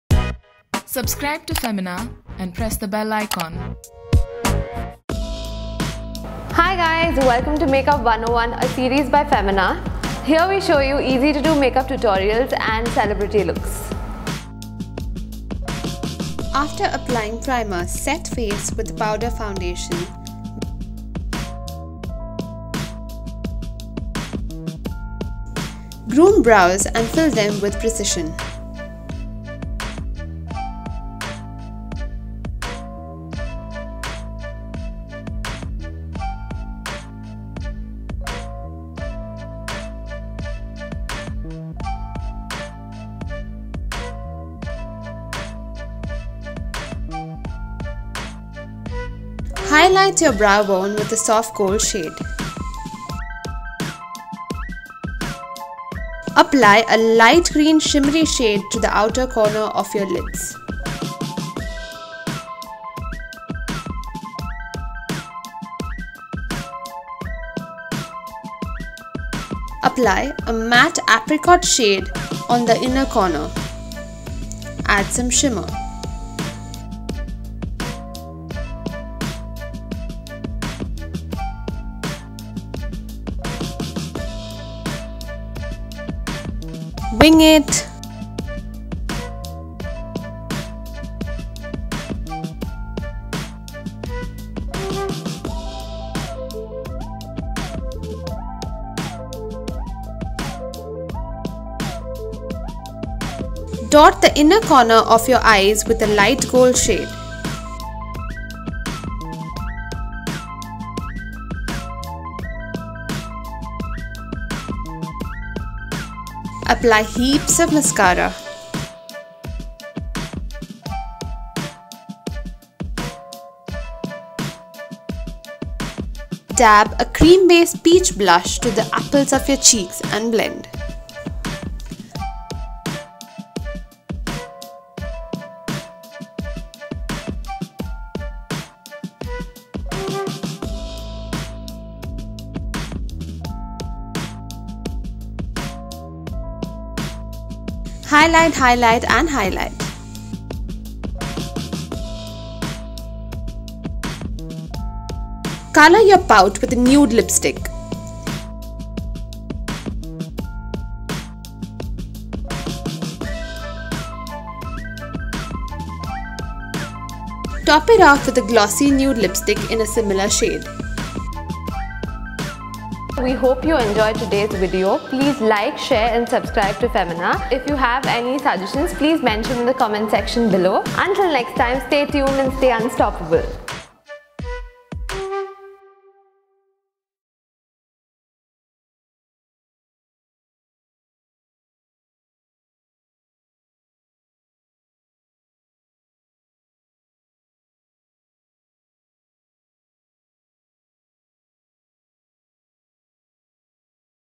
Summer Makeup Tutorial | Easy Everyday Makeup Look | Makeup 101 | Femina Beauty
This easy-breezy summer makeup look is far from plain and has a delicious pop of colour that you will love. Try this tropical-inspired look at home and get ready for those days we can step out and drink in the sunshine.

Welcome to Makeup 101. Here we show you how to nail the latest beauty trends and how to look glamorous all day long.
Format: Video and video story on website